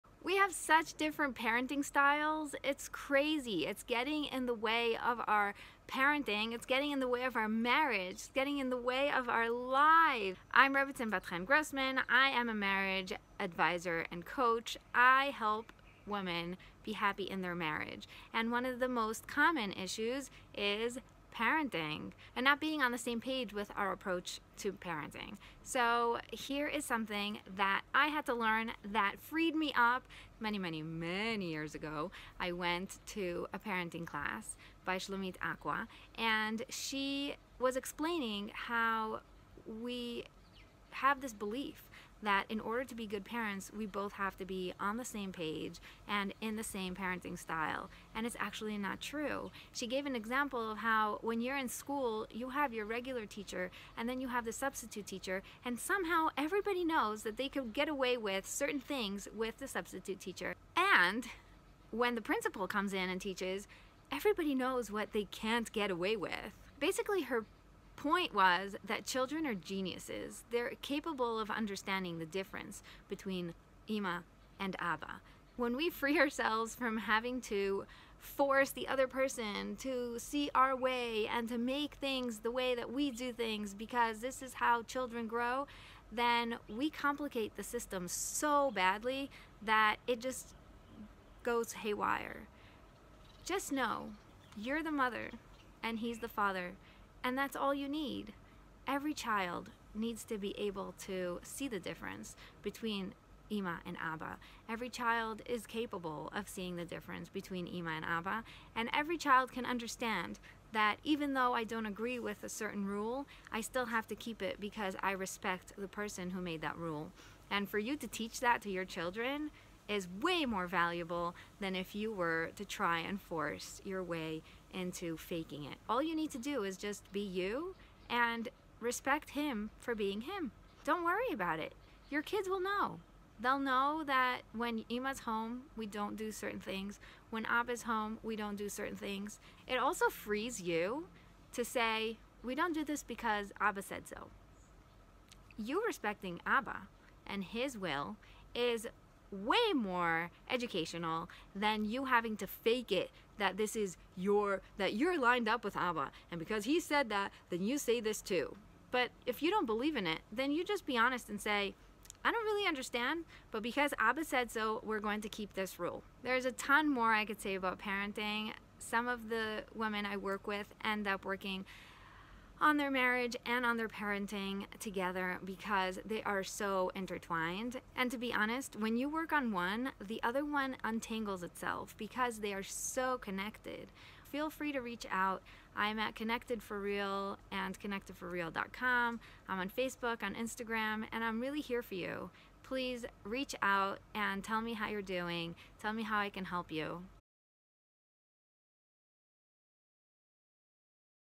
Parenting Styles & Marriage | ConnectedForReal
"We have such different parenting styles, it gets in the way of our marriage!"
Chinuch is the number one source of frustration in a Jewish marriage and I want you to know it doesn't have to be! Watch how my parenting teacher many years ago freed me from the belief that we need to be lined up and on the same page in regards to our parenting styles. This changed my life!